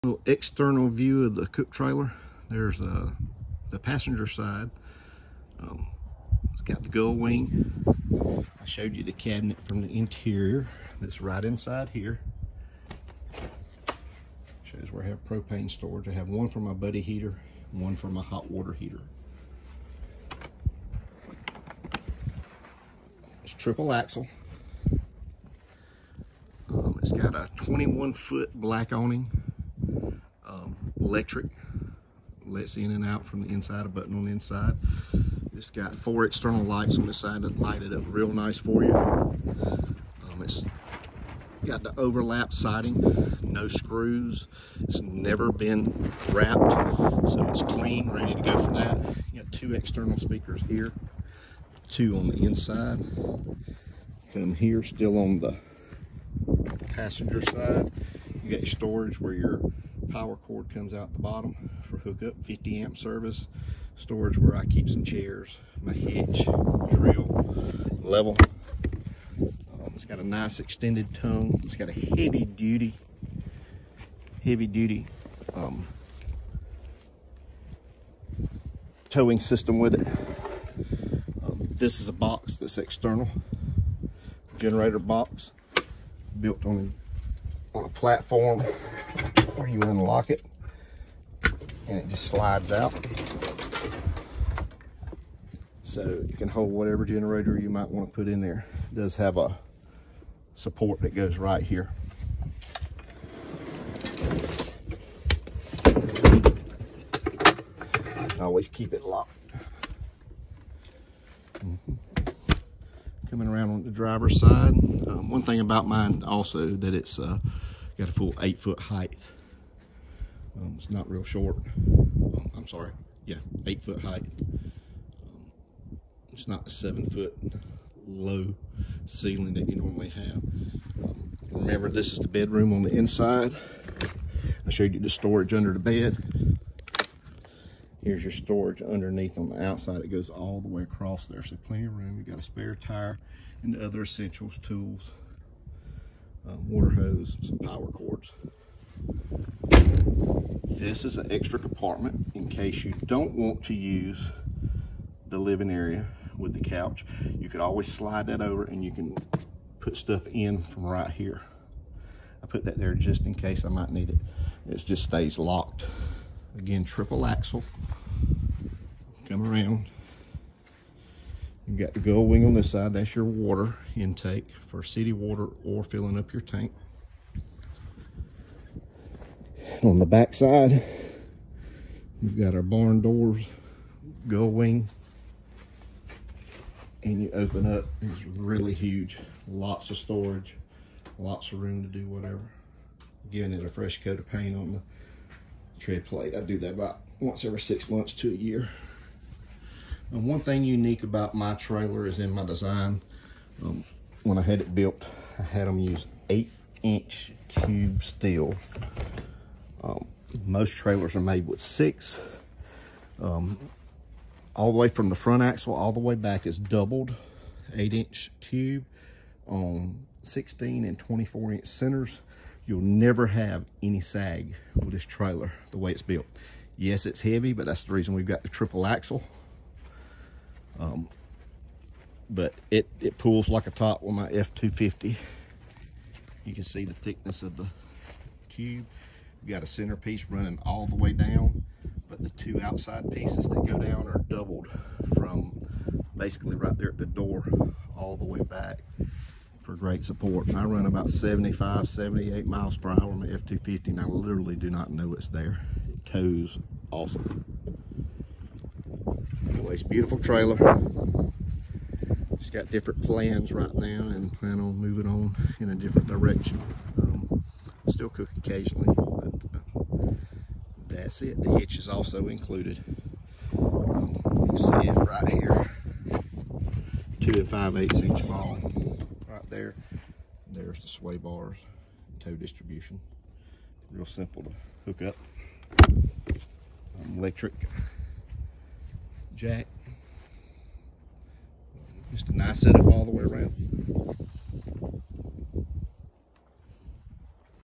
2019 Freedom Trailers Review - BBQ Cook Trailer - Enclosed Trailer - Cargo Trailer - Exterior Setup
The Perfect Competition BBQ Cook Trailer - 2019 Freedom Trailer - Exterior Setup
2019 Freedom Cargo Trailer Review - The Perfect Competition BBQ Cook Trailer - Exterior Setup
Freedom built my cargo trailer to my exact specifications, amazing interior cabinetry, trim, door design, lighting, and quality!

Freedom Trailers, LLC
774 W Marshall Ave.